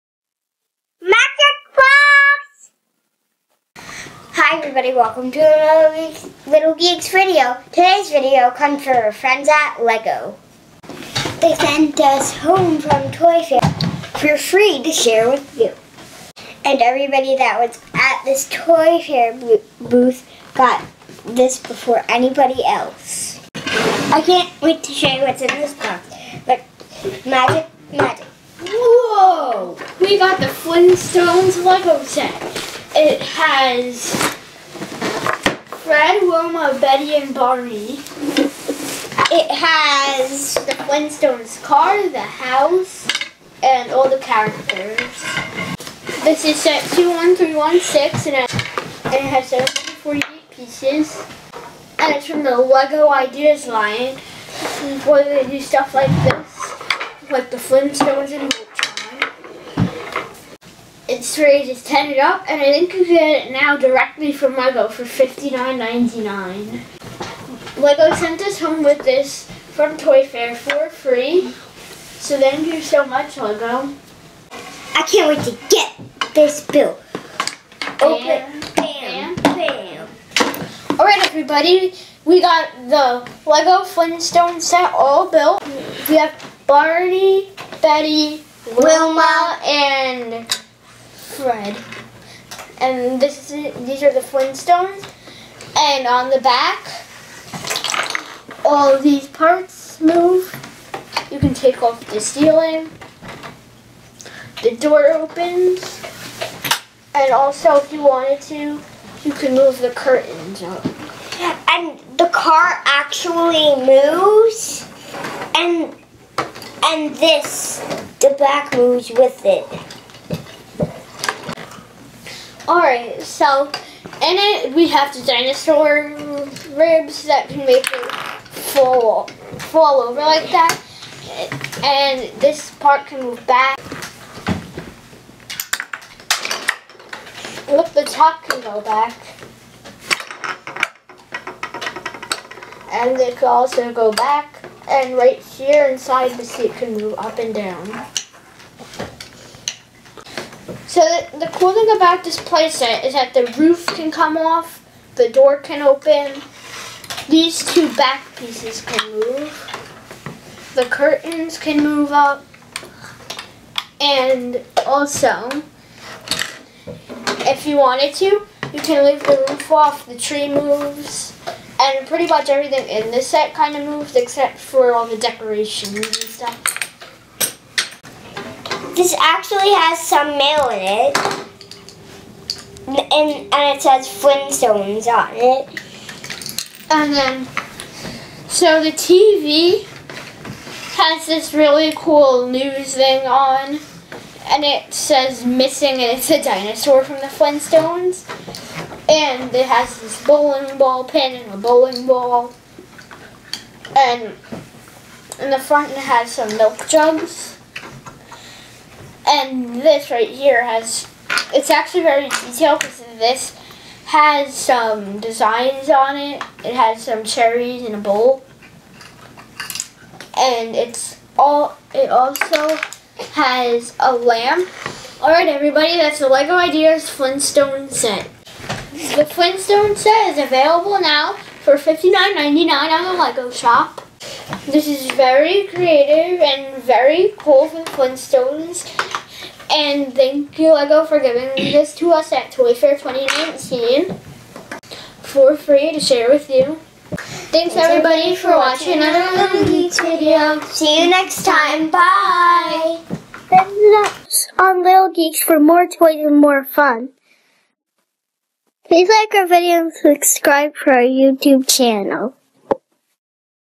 Magic Box: LEGO Ideas The Flintstones
The litlgeeks got their hands on the brand new The Flintstones LEGO set from LEGO Ideas, from our friends at LEGO, from Toy Fair 2019!  Let's check it out!

 This product is for ages 10 and up.
The litlgeeks received this product for free for review purposes.
Please excuse litlninja's speech, he is learning to speak through a palate expander.